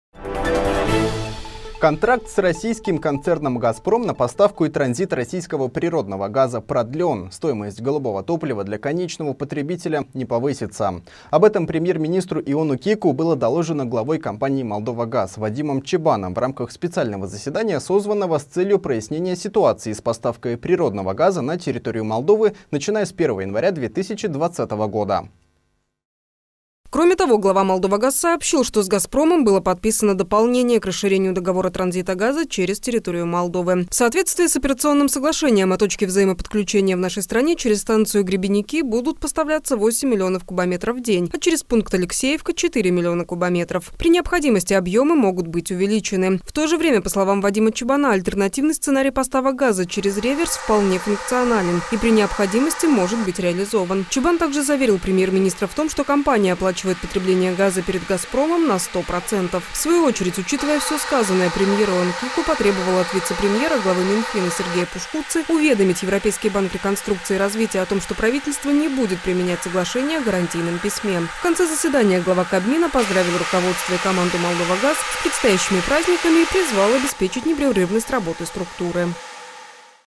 Контракт продлен! Цена на газ не вырастет!
Контракт с российским концерном «Газпром» на поставку и транзит российского природного газа продлен, стоимость голубого топлива для конечного потребителя не повысится. Об этом премьер-министру Иону Кику было доложено главой компании «Молдовагаз» Вадимом Чебаном, в рамках специального заседание для прояснения ситуации с поставкой природного газа на территорию Республики Молдова, начиная с 1 января 2020 года.